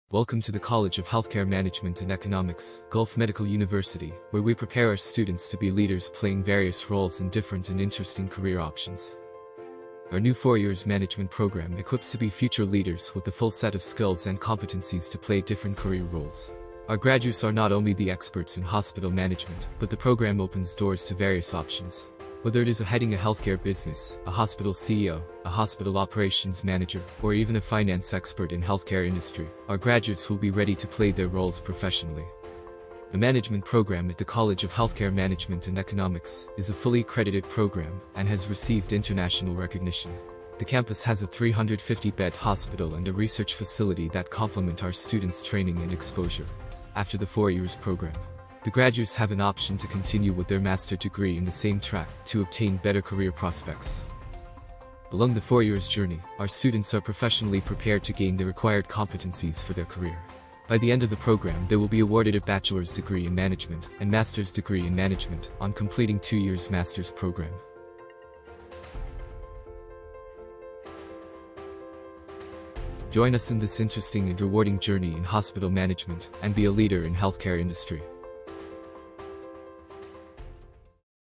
College of Healthcare Management and Economics
The College of Healthcare Management and Economics (CoHME) is leading the charge into the next generation of healthcare management education.

The new college of Healthcare Management and Economics (HME) allows GMU to maintain its differentiation strategy in the health industry in higher education and fill the acute shortage of healthcare management professionals. The healthcare industry is the second largest sector in most countries. The healthcare sector in the region is vibrant and thriving-constantly driving continued growth across all segments.

The new college of Healthcare Management and Economics was established to offer two unique programs that leverage the experience of the existing medical faculty and the large network of healthcare facilities affiliated to GMU.

The college is offering a four years Bachelor of Science in Healthcare Management and Economics and a 3 Semesters Executives Master in Healthcare Management and Economics.